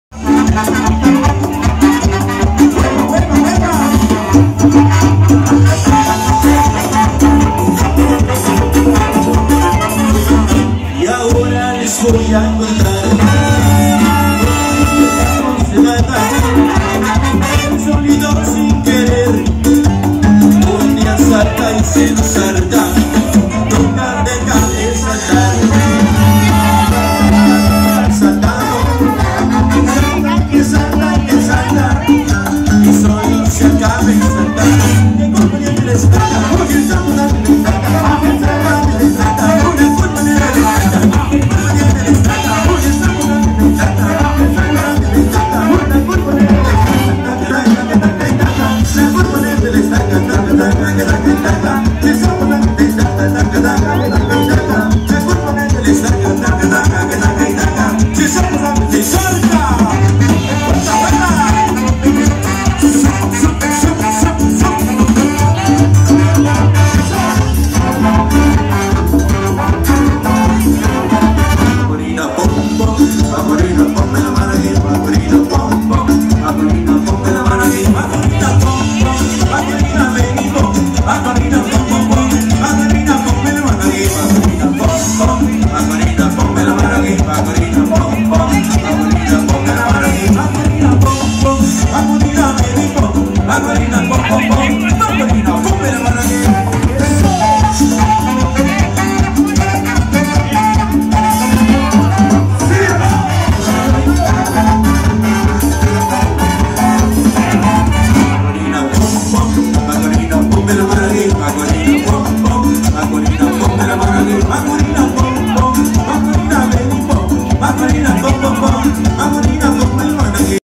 Súper Grupo Dimensión Latina en vivo, Junio del 2022
En vivo desde la Colonia Andrés Quintana Roo municipio de Jiquipilas, Chiapas!
Originarios de Cintalapa, Chiapas México!